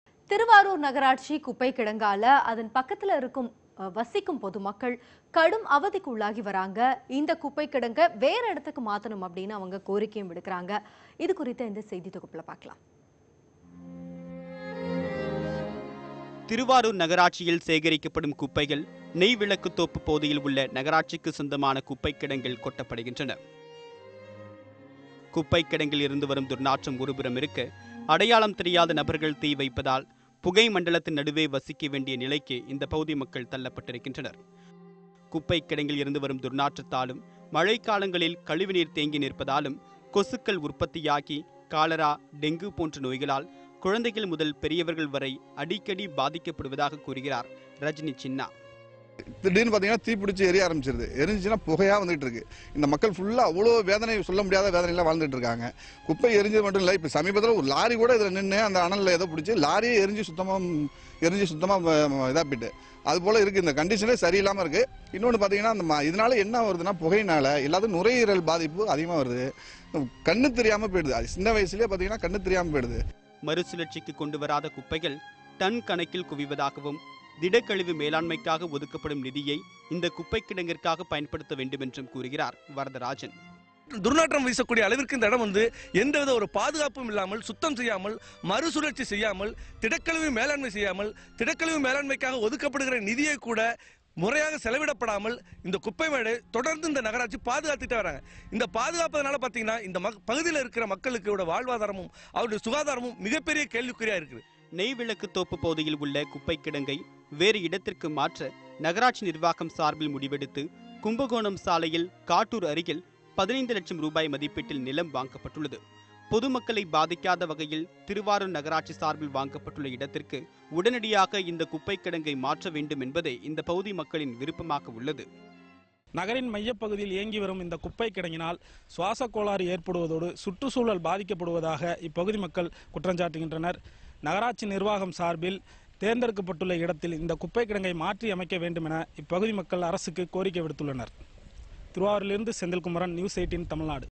குப்பைக்கிடங்கை மாற்ற கோரிக்கை...
திருவாரூர் நகராட்சி குப்பைக் கிடங்கால் அதன் அருகே வசிக்கும் பொதுமக்கள், கடும் அவதிக்குள்ளாகி வருகின்றனர். இந்த குப்பைக்கிடங்கை  வேறு இடத்திற்கு மாற்ற பொதுமக்கள் கோரிக்கை